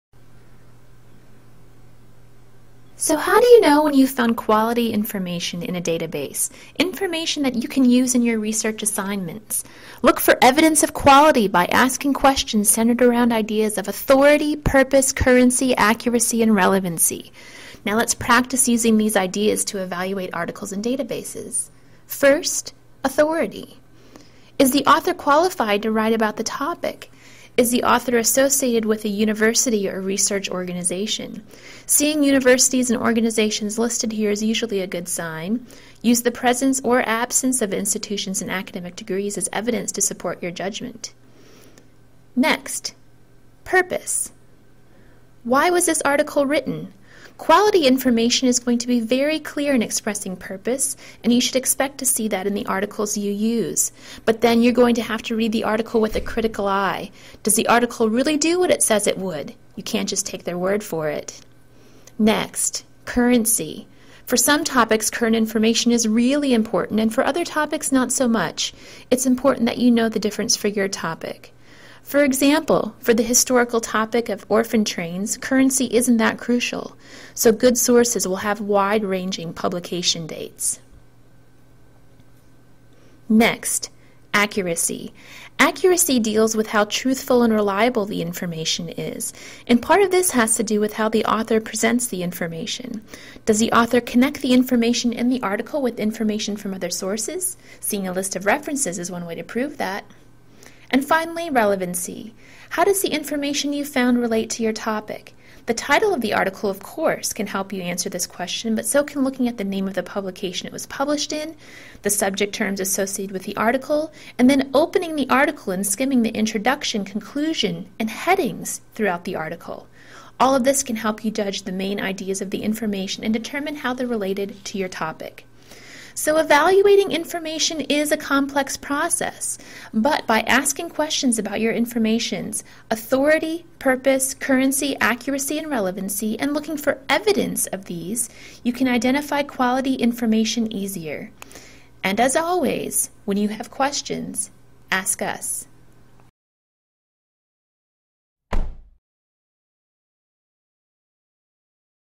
UK 101 2016: Evaluating Database Results
How to recognize quality information in database results connected to a topic from the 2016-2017 Common Reading, Orphan Train, for UK 101 students at the University of Kentucky